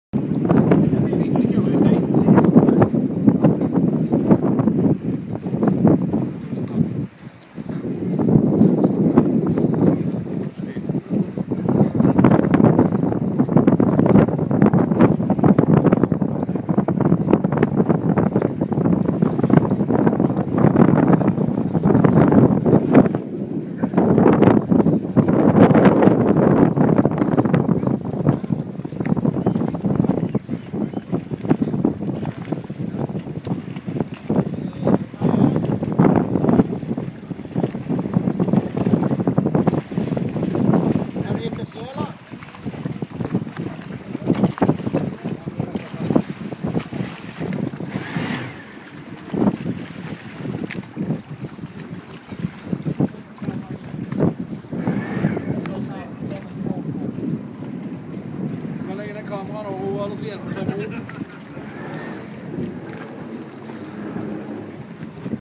Marex 320 ACC beide
Eerste varende beelden van de Marex 320 Aft Cabin Cruiser - juni 2012 - Fevik, Norway